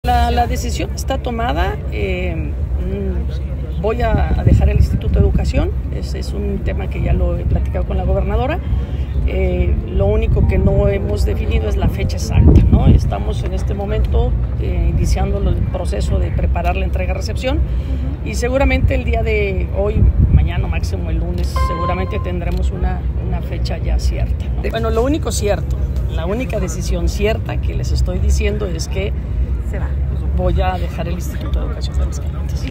Confirma Lorena Martínez que dejará el IEA
* No se ha definido fecha exacta
* No confirma ni niega si contendería por la alianza de Morena

Aguascalientes, Ags. 10 enero 2023.- La directora del Instituto de Educación de Aguascalientes, Lorena Martínez Rodríguez confirmó que dejará el cargo en el transcurso de los próximos días, si bien aún no ha definido la fecha exacta.
Durante los últimos días del año pasado, se haba puesto públicamente el 15 de enero como límite para tomar una determinación.
Hoy, en entrevista colectiva, dio a conocer que ha informado ya a la Gobernadora Tere Jiménez que dejará la titularidad del IEA, para lo cual prepara ya la entrega-recepción.
A Lorena Martínez se le liga con la alianza de Morena, vía el PVEM para competir por una senaduría, hecho que tampoco quiso confirmar ni negar hoy.